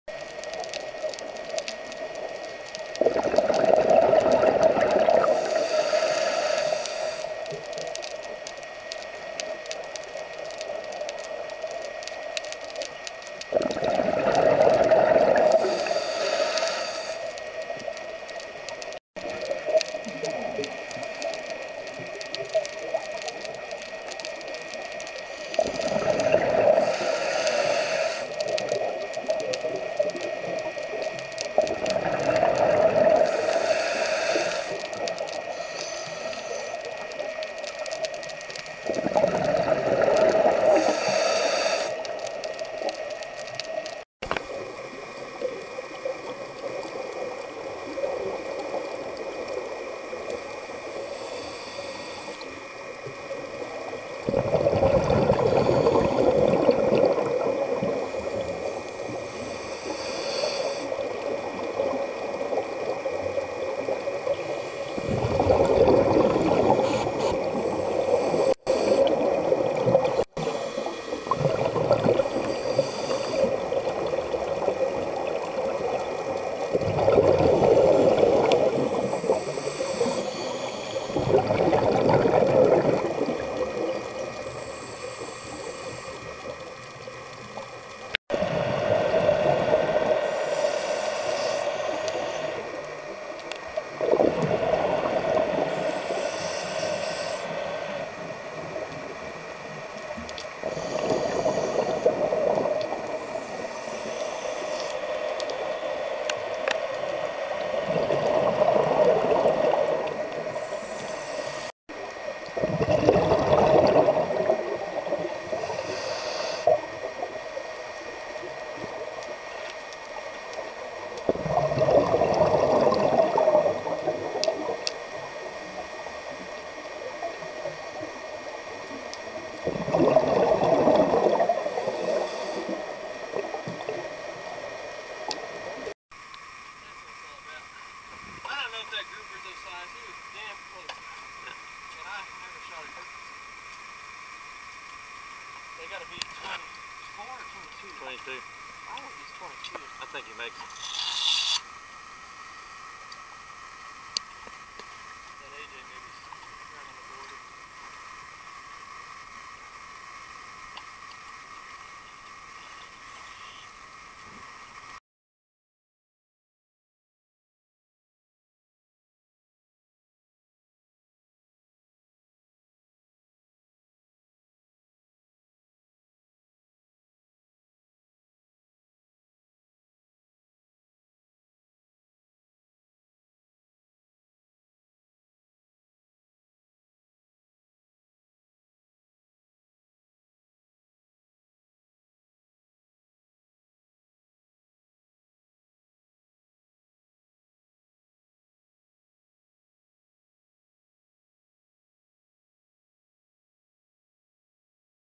Killing Cudas for Dinner
Diving in P.C. FL and shooting big Barracudas for a fish fry.